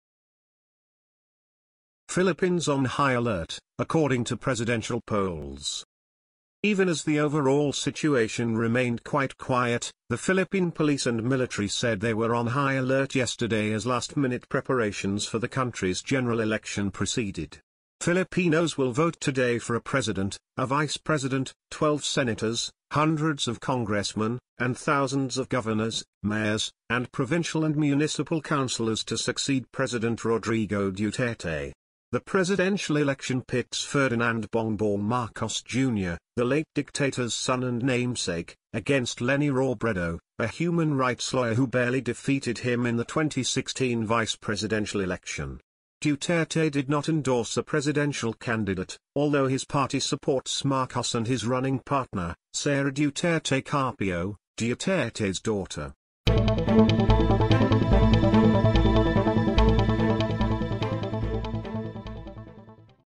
Philippines on high alert, according to presidential polls | NewsRme
Even as the overall situation remained "quite quiet," the Philippine police and military said they were on high alert yesterday as last-minute preparations for the country's general election proceeded. Filipinos will vote today for a president, a vice president, 12 senators, hundreds of congressmen, and thousands of governors, mayors, and provincial and municipal councillors to succeed President Rodrigo Duterte. The presidential election pits Ferdinand "Bongbong" Marcos Jr., the late dictator's son and namesake, against Leni Robredo, a human rights lawyer who barely defeated him in the 2016 vice presidential election. Duterte did not endorse a presidential candidate, although his party supports Marcos and his running partner, Sara Duterte-Carpio, Duterte's daughter.